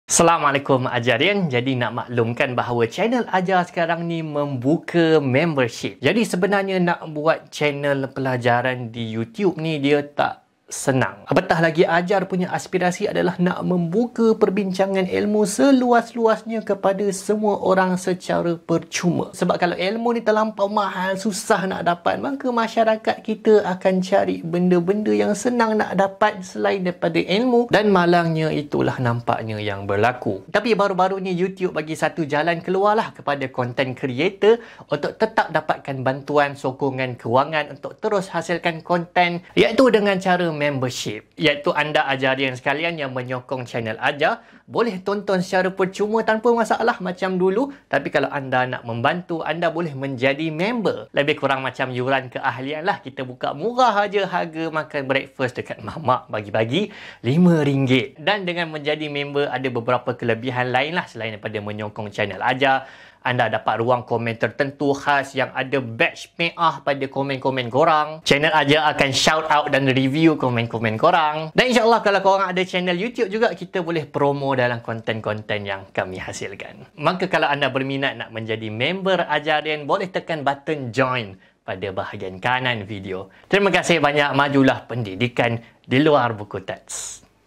Jom Jadi Ahli Ajarian Youtube! :D
Harapan channel AJAR adalah nak membuka seluasnya capaian content pelajaran dan ilmu secara PERCUMA, tanpa perlu bayar untuk tengok, tanpa paywall, dan tak ekslusif, hanya beberapa orang je boleh tengok.

Sebab kalau content pelajaran susah dicapai, mahal, masyarakat akan cari content lain selain daripada pelajaran yang sangat senang dapat...

Alhamdulillah, Youtube sekarang membuka butang keahlian untuk content creator. Anda yang berminat untuk menyokong channel AJAR boleh menjadi AJARIAN dengan memberikan sedikit sokongan kewangan (yang sebenarnya lebih simbol sokongan kalian ^^)

Sudah pasti, dengan menjadi AJARIAN, anda dapat kelebihan istimewa semacam badge PEAH pada komen korang (semakin lama jadi member, makin banyak badge!), shoutout komen - komen korang dan AJAR boleh review content pelajaran korang!

Insya Allah selepas ini kita akan kembangkan lagi fungsi membership. Mungkin buat meetup bersama, buat tutorial dan sebagainya (apa cadangan korang? Tulis di komen ya :)

Terima kasih atas sokongan selama ini :D Majulah AJARIAN, pendidikan luar buku teks ^^